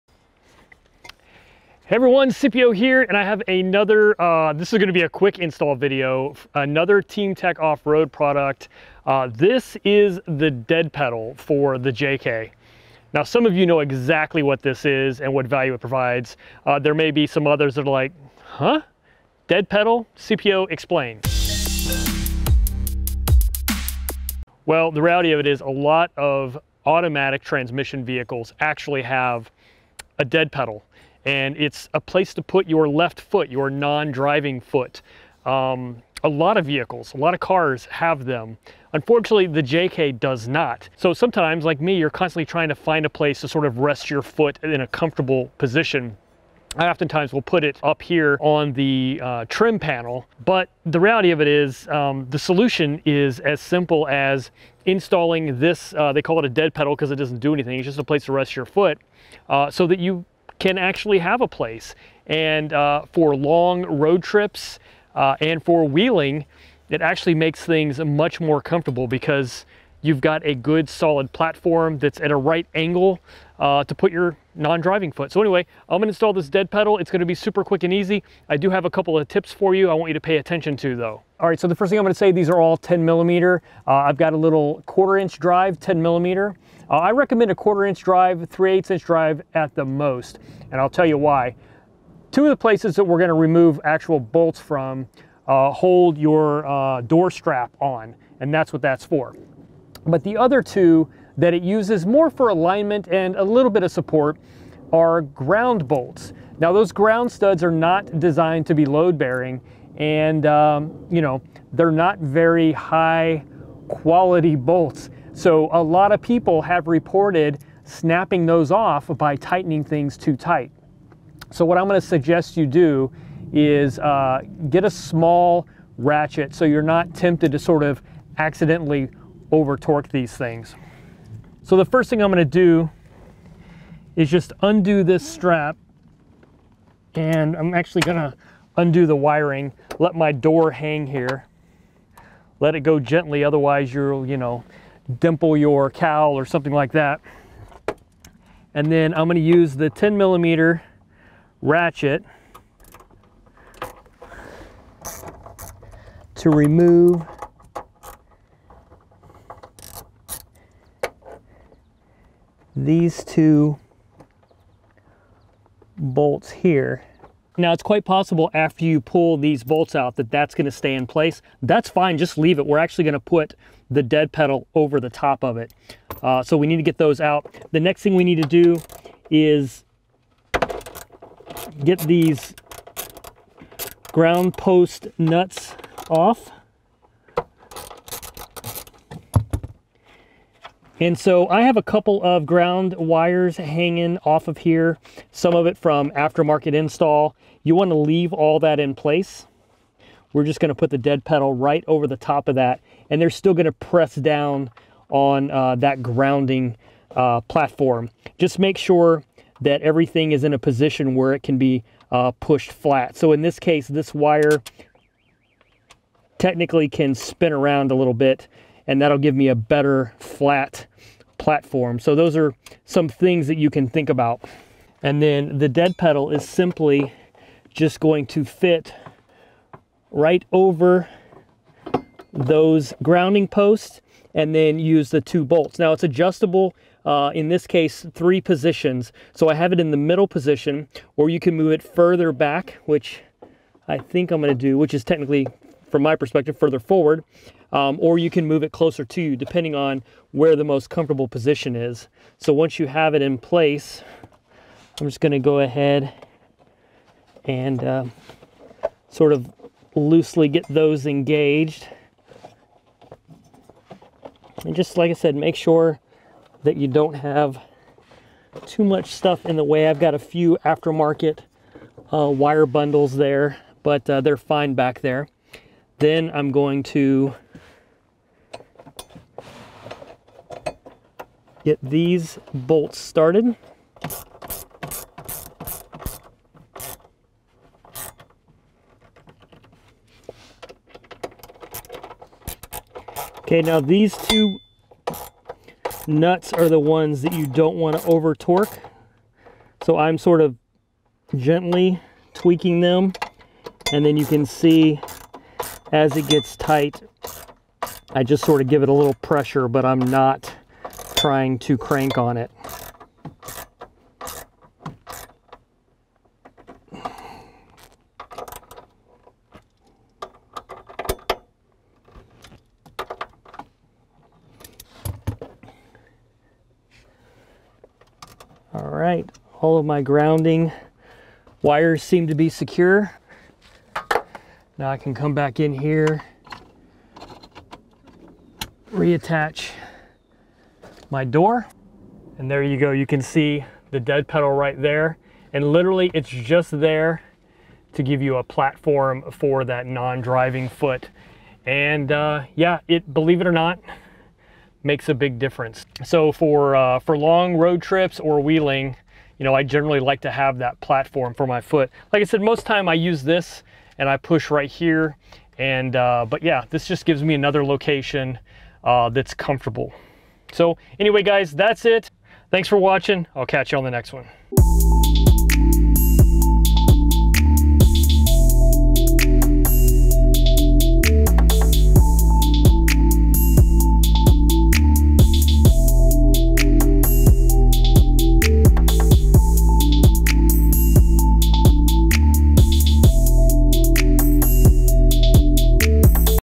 JK Dead Pedal Install from Team Tech Offroad
TTO DEAD PEDAL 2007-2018 JEEP JK BLACK POWDER COATED LEFT SIDE FOOT REST

-Durable and heavy duty design completes the interior of your Jeep Wrangler with this Dead Pedal for left side foot rest. This new easy bolt-in Dead Pedal helps with leg fatigue that many of us get while driving when you do not have a place to support your left foot. Now, you can firmly plant your foot with confidence in the most extreme off road driving conditions or take a much needed load off in rush-hour traffic. The added comfort and safety of our Dead Pedal makes it the perfect addition for any daily driver or weekend warrior.

- Easy to install. The installation only takes a few minutes and basic hand tools to complete the job. No Drilling Required.

- Featuring a non-slip design, and a textured powder coated finish for excellent durability and aesthetics.

- Black rugged coating  - guaranteed not to rust.

- Fitting: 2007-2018 Jeep JK All Automatic Transmission Models.